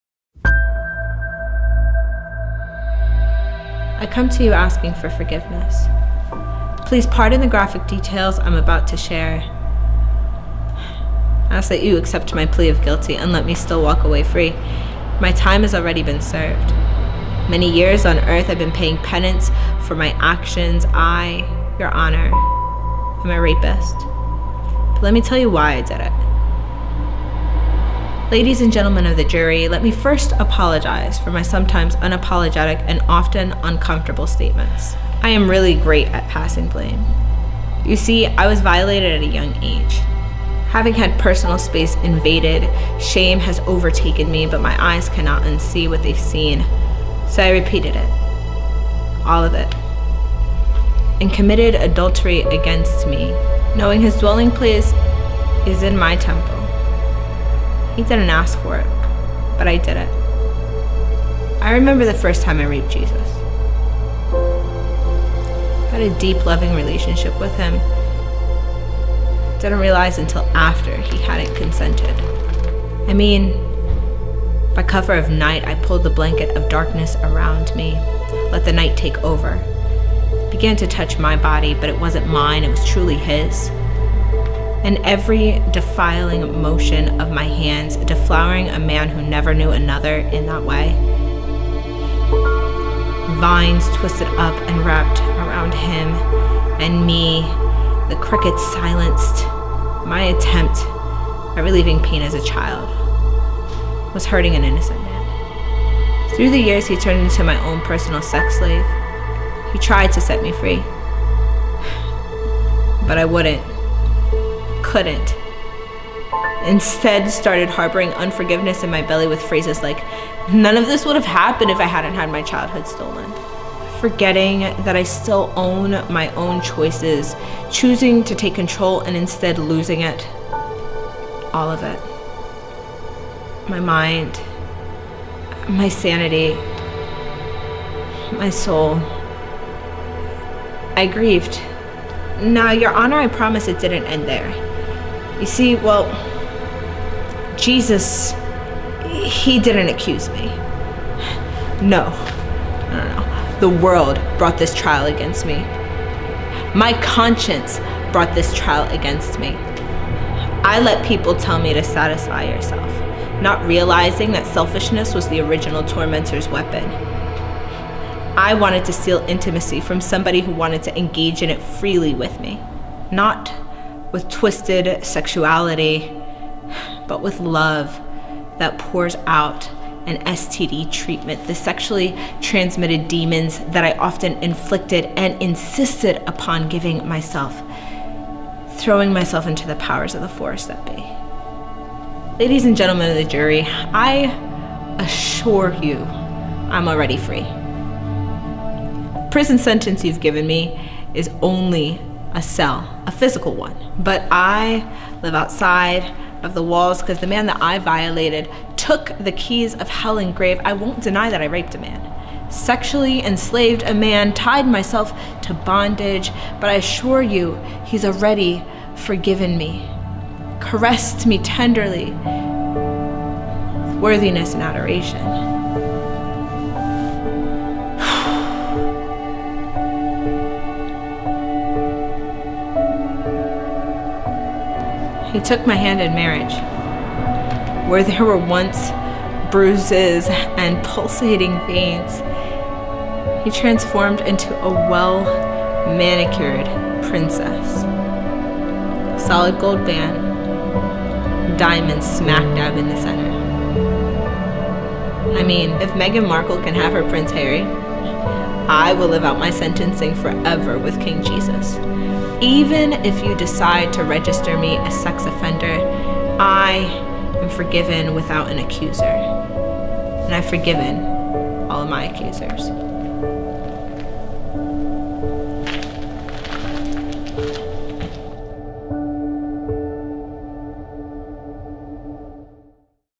Raping Jesus by Gi the Poet
Spoken word piece by Gi the Poet

In a world where selfishness and feeding on your own fleshly desires creates hurt on the inside because we are not truly satisfied fully, Gi explores the idea of admitting guilt to a crime that could change the landscapes of our lives forever.

Filmed, edited, directed, and produced by: Khristian Mayfield
Written by: Gi the Poet